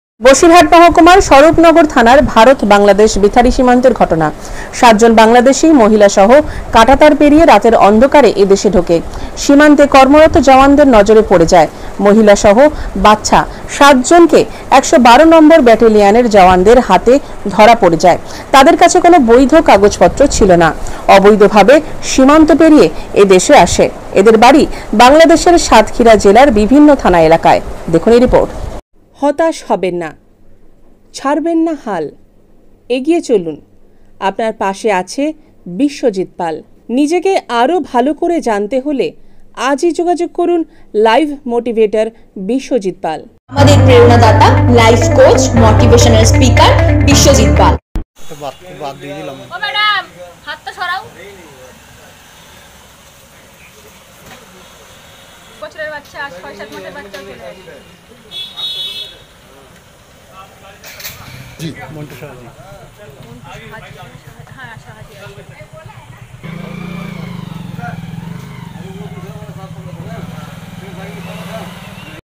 ভারত-বাংলাদেশ সীমান্ত থেকে, বাংলাদেশি ৭ মহিলা গ্রেফতার
বসিরহাট মহাকুমার স্বরূপনগর থানার ভারত-বাংলাদেশ বিথারী সীমান্তের ঘটনা। সাতজন বাংলাদেশী মহিলাসহ কাঁটাতার পেরিয়ে রাতের অন্ধকারে এ দেশে ঢোকে । সীমান্তে কর্মরত জওয়ানদের নজরে পড়ে যায় মহিলাসহ বাচ্চা ৭ জনকে ১১২ নম্বর ব্যাটালিয়নের জওয়ানদের হাতে ধরা পড়ে যায়, তাদের কাছে কোন বৈধ কাগজপত্র ছিল না। অবৈধভাবে সীমান্ত পেরিয়ে এ দেশে আসে‌ এদের বাড়ি বাংলাদেশের সাতক্ষীরা জেলা বিভিন্ন থানা এলাকায়। এই বাংলাদেশি মহিলারা কি কারণে ভারতে ঢুকে ছিল তার সঠিক উত্তর  দিতে পারেনি তার পর এদেরকে গ্রেফতার করে বিএসএফ।ওই ৭ জনকে বিএসএফের পক্ষ থেকে স্বরূপনগর থানার পুলিশের হাতে তুলে দেওয়া হয়।  ধৃত সাত বাংলাদেশিকে আজ বৃহস্পতিবার বসিরহাট মহকুমা আদালতে তোলা হবে।